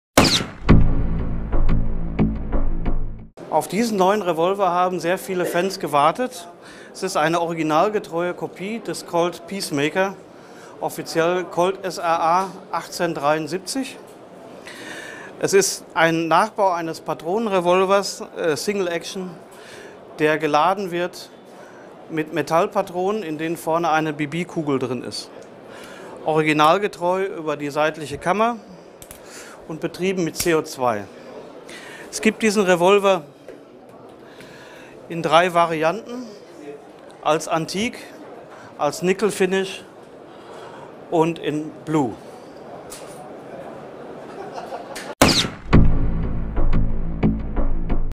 Colt Peacemaker: Wie gut ist die CO2-Druckluftreplika von UMAREX?
Weitere Informationen zur CO2-Luftpistole Colt Peacemaker von UMAREX findet ihr

Ulrich Eichstädt von UMAREX stellt euch die Replika-Version des Colt Peacemaker vor. Dabei handelt es sich um eine originalgetreue Nachbildung des berühmten Revolvers aus dem Wilden Westen als CO2-Luftpistole. Der Peacemaker von UMAREX wurde für das Plinking, den Schießstand, preiswerte Trainingseinheiten sowie zum Heranführen von jüngeren Schützen an das Westernschießen konzipiert. Mehr zu dem Revolver erfahrt ihr im Video!